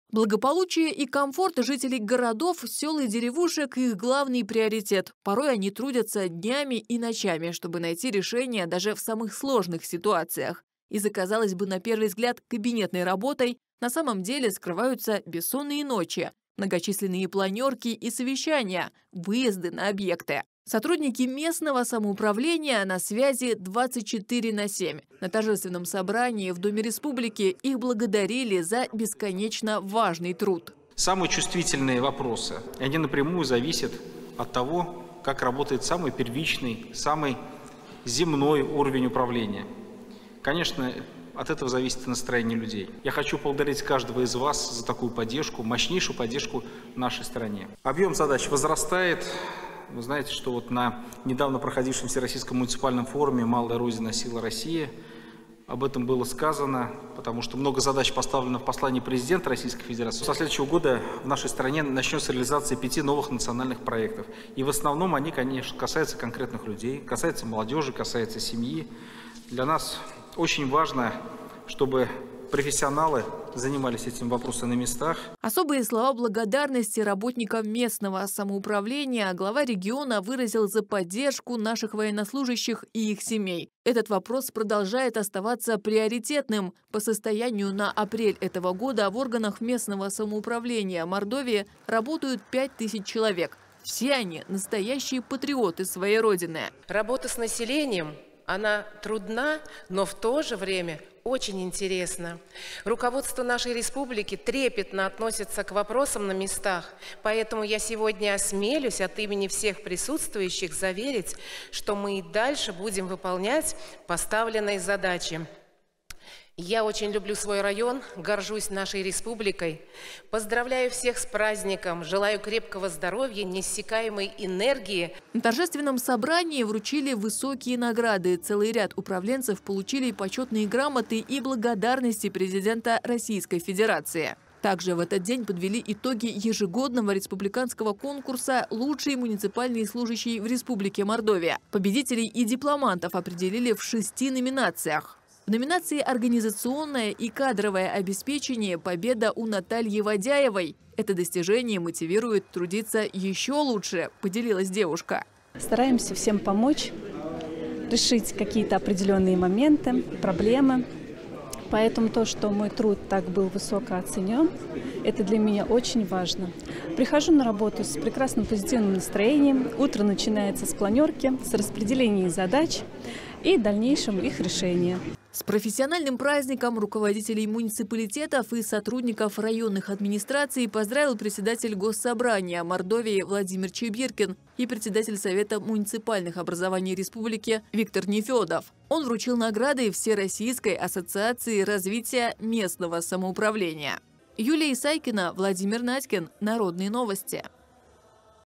В Доме республики чествовали работников местного самоуправления
Намного больше, чем просто управленцы. Те, кто решают важнейшие задачи, работают ради благополучия людей — каждый на своём месте. В Доме республики состоялось торжественное собрание, посвящённое Дню местного самоуправления. С профессиональным праздником руководителей муниципалитетов и сотрудников районных администраций поздравил Глава Мордовии Артём Здунов. Он поблагодарил их за труд и вручил высокие награды. Подробности в сюжете Юлии Исайкиной и Владимира Надькина.